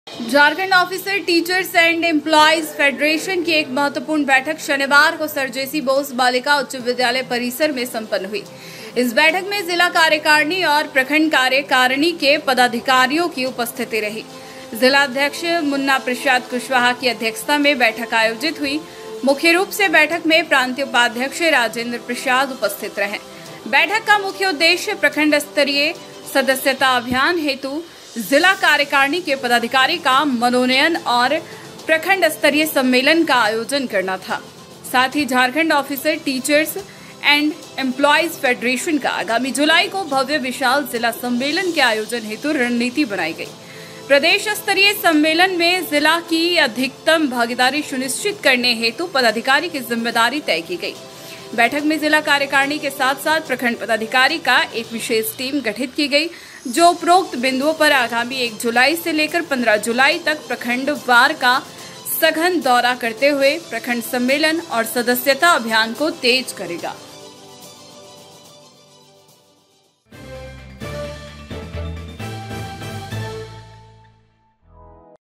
झारखंड ऑफिसर टीचर्स एंड एम्पलाइज फेडरेशन की एक महत्वपूर्ण बैठक सर जेसी बोस बालिका में हुई संपन्न
झारखंड ऑफिसर टीचर्स एंड एम्पलाइज फेडरेशन की एक महत्वपूर्ण बैठक सर जेसी बोस बालिका उच्च विद्यालय परिसर में हुई संपन्न